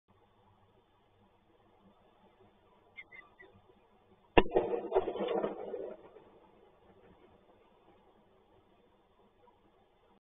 Sokół wędrowny PGE Toruń - Huk wystrzału płoszy rezydentów 😤
Filmik nagrany dzięki uprzejmości
Peregrinus. pl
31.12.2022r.